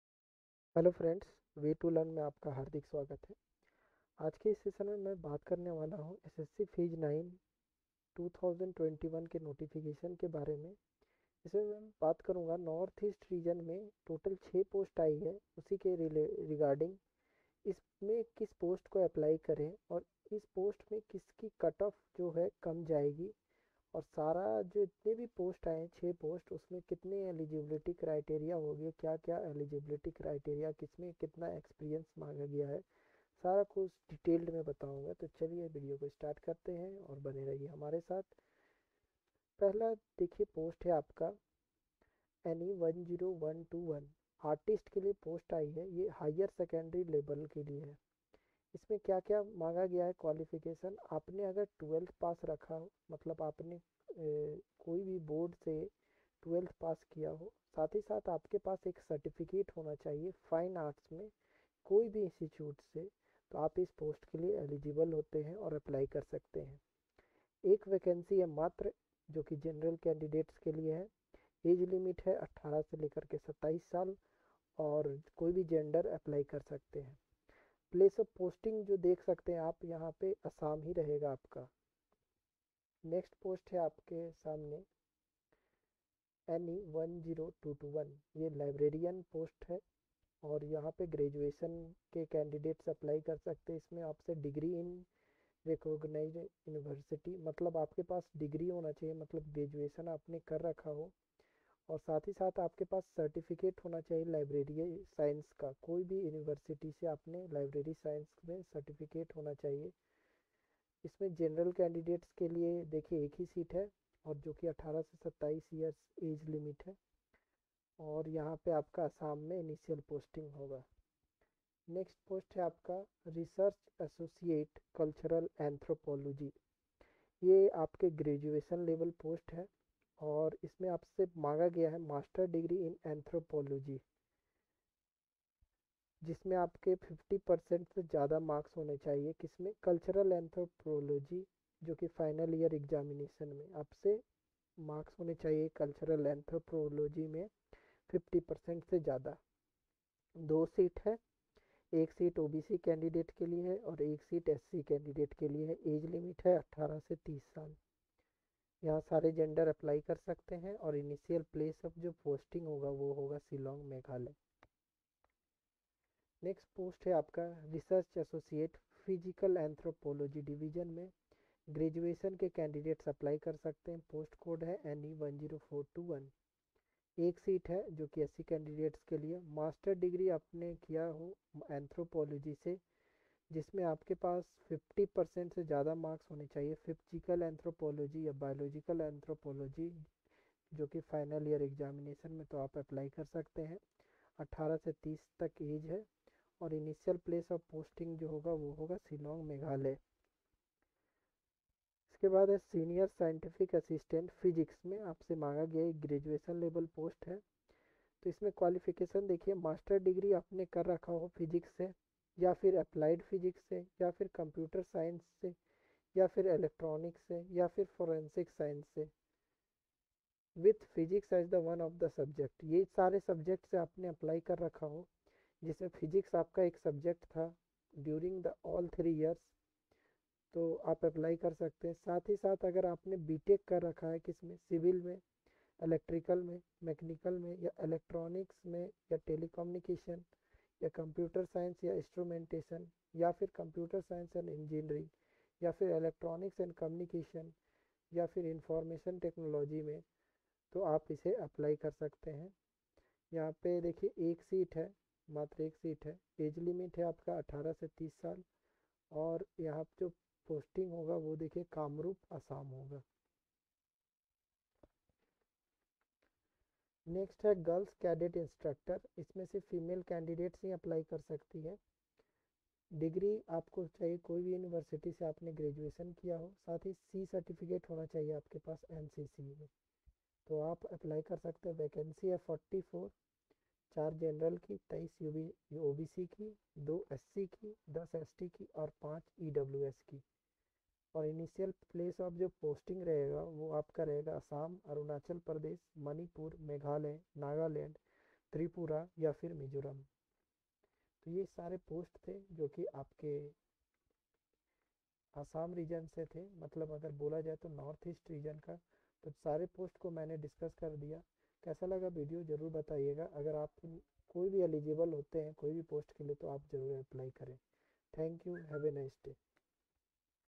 SSC PHASE IX 2021 NOTIFICATION|NORTH EAST REGION |All Post Eligibility Criteria |6 posts
In this Video , I will discuss about the eligibility criteria of all the post that are  in ssc phase IX notification,
I described about the experience details of the post .
I think you will like my video